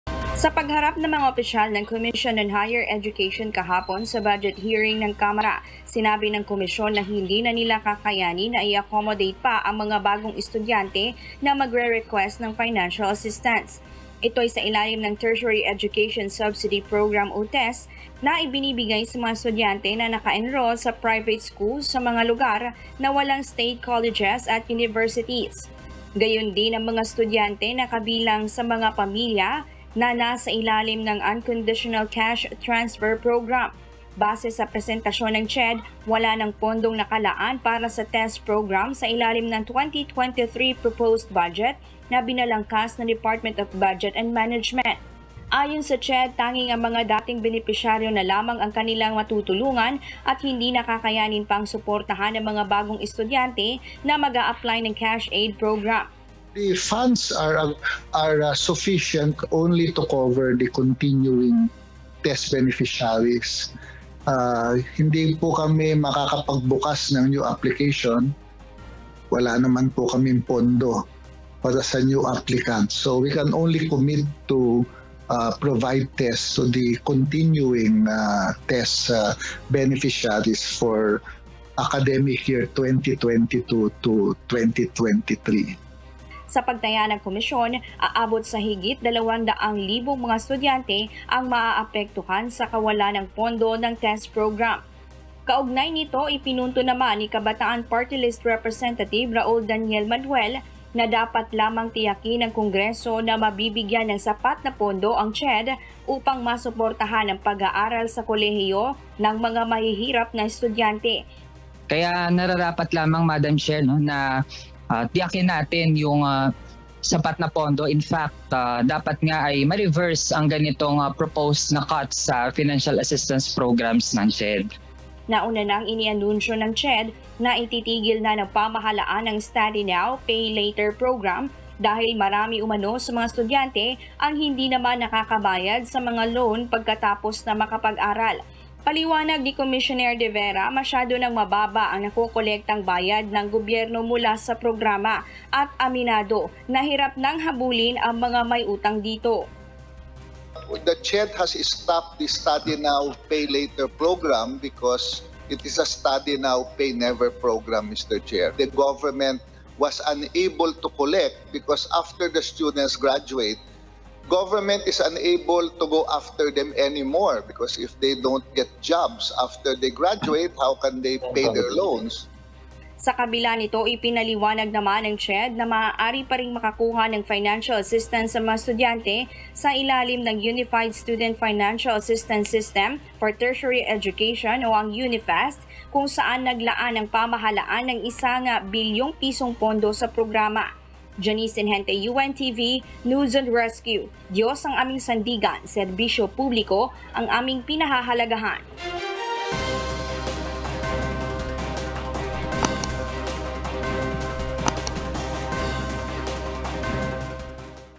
CHED, wala nang pondo para sa mga bagong aplikante sa college education aid program
Hindi na umano kakayanin ng pondo ng Commission on Higher Education na i-accommodate ang mga bagong estudyanteng mag-aapply ng scholarship ngayong pasukan.

Una nang inianunsyo ng komisyon na ipinatigil na nito ang ‘Study now, pay later’ program dahil maraming benepisyaryo ang hindi nakababayad.